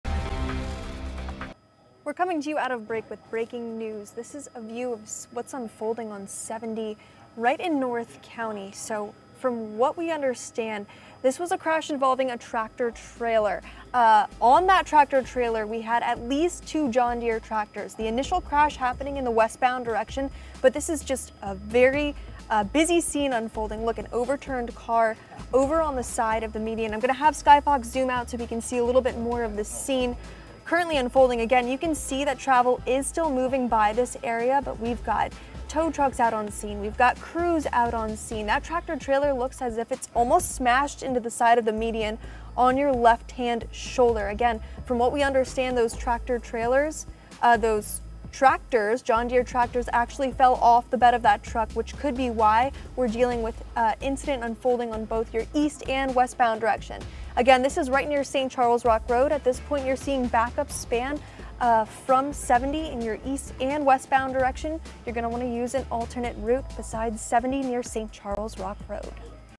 Accident at 70 and St. Charles Rock Road
Bommarito Automotive SKYFOX is above an accident at 70 and St. Charles Rock Road, where a tractor trailer lost some equipment, and there have also been reports of a few overturned cars.